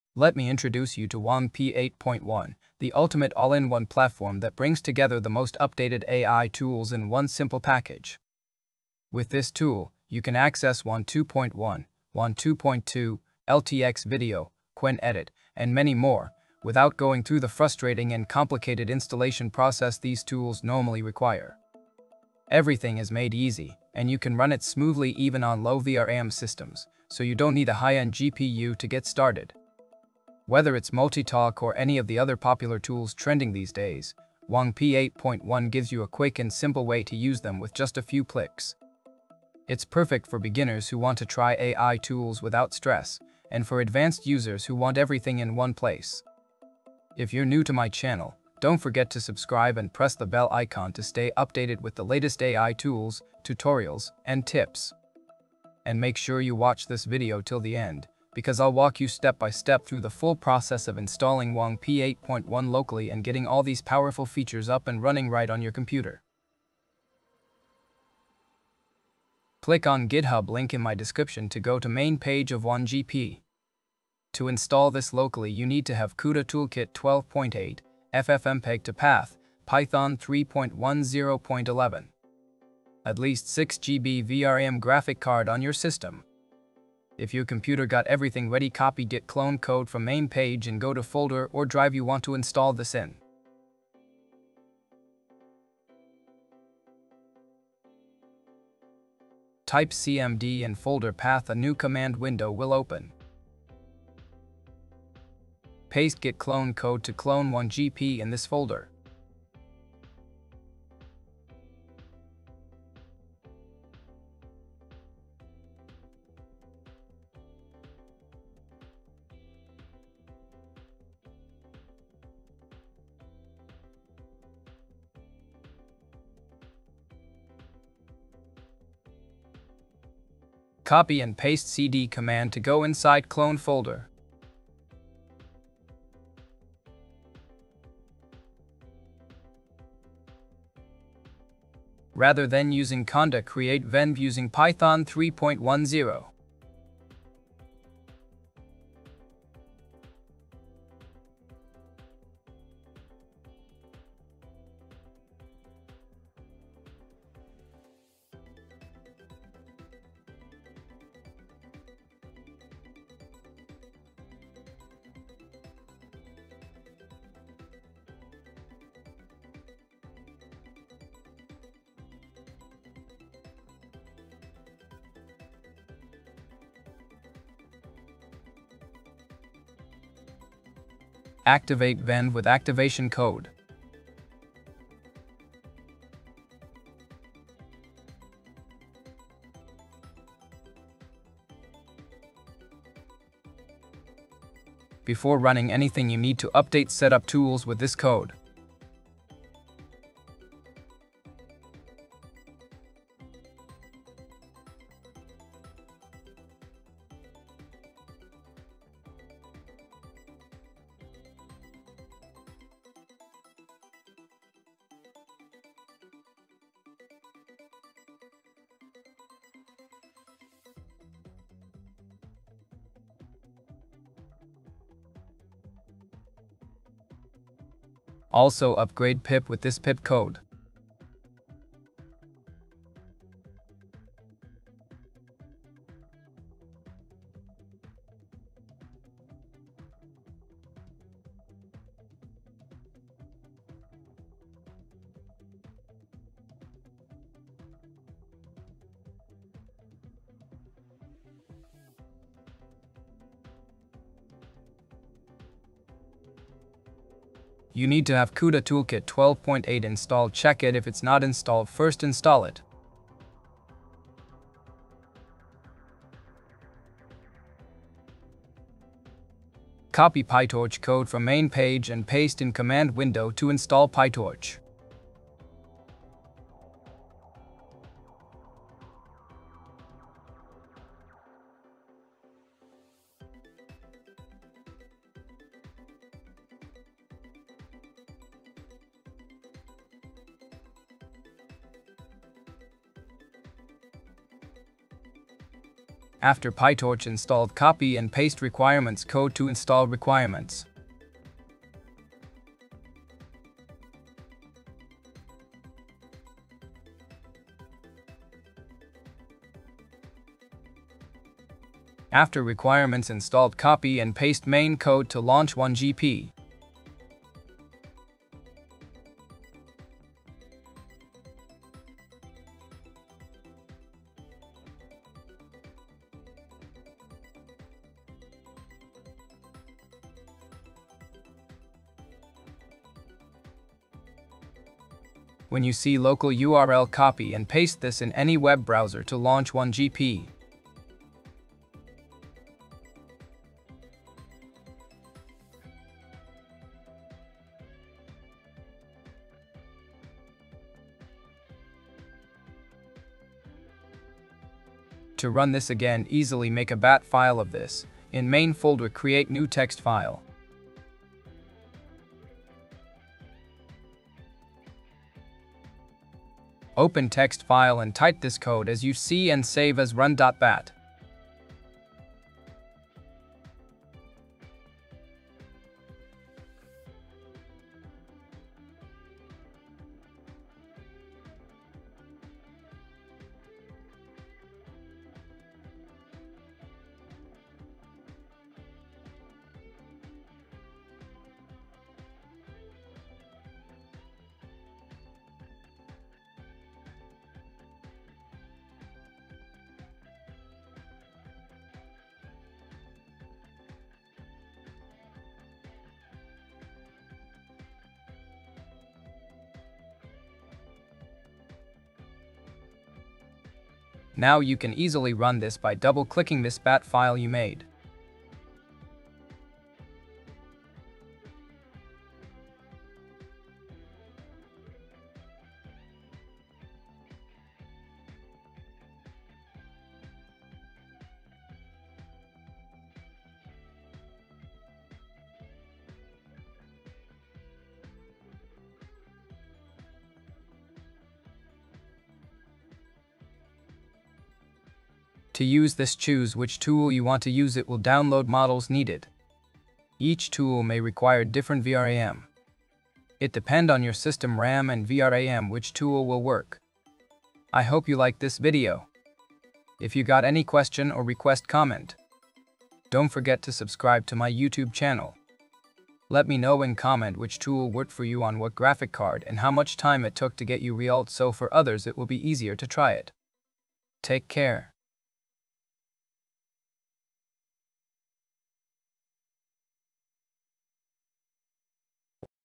All AI Tools in ONE Place! (Wan 2.2, LTX, InfiniteTalk & More) | Local Installation Guide (Low VRAM)
🚀 Want to use all the latest AI tools in one place without complicated installs? In this video, I’ll show you how to set up WangP 8.1 locally and run Wan 2.2, LTX Video, Qwen Edit, InfiniteTalk and more — all on Low VRAM PCs. No errors, no hassle, just one simple tool!